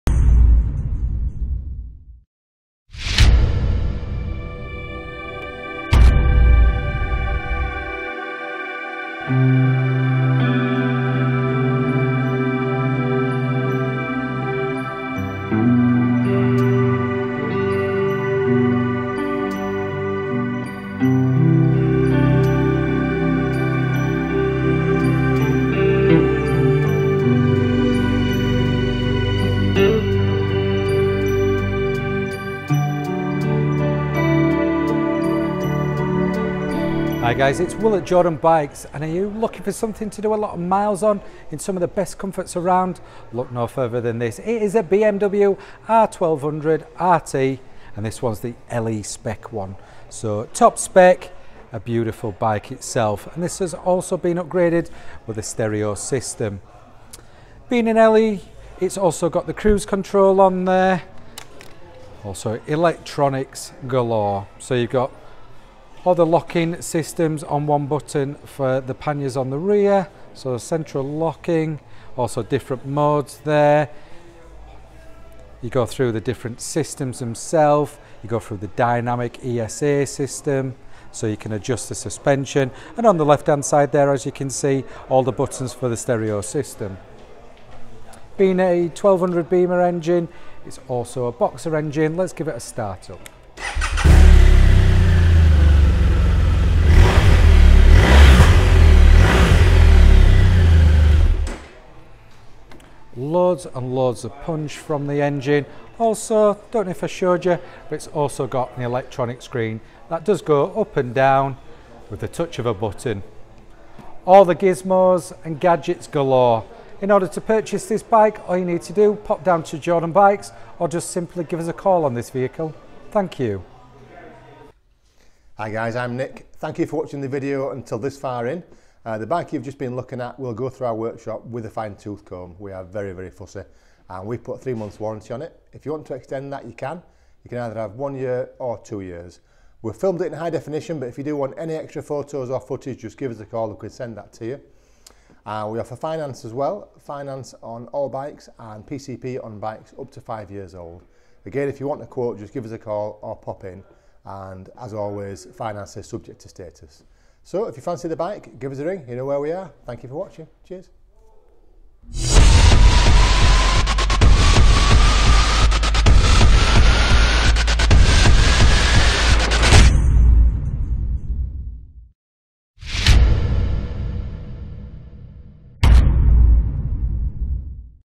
Jordanbikes- For Sale, BMW R1200RT LE ABS, 2016 65 plate, 14,184 miles, £9990
For Sale, BMW R1200RT LE ABS, 2016 '65' plate with only 14,184 miles on the clock. This is an absolutely stunning bike that has clearly been well cared for. The R1200RT is the ultimate tourer offering superb comfort and the ability to eat up the miles with ease. As standard they come with ABS, traction control, electric screen and rider modes. This is the range topping LE model adding dynamic riding modes, ESA Electronic Suspension Adjustment, cruise control, heated grips, heated rider and passenger seats, satellite navigation preparation and dynamic traction control. This particular bike has been further enhanced with audio system, and the Dynamic Pack with LED running lights and tyre pressure monitoring. This really has everything you could ever want and will be leaving us after a service and a fresh MOT so will want for nothing for a long time and be ready to be enjoyed. Yours for only £9990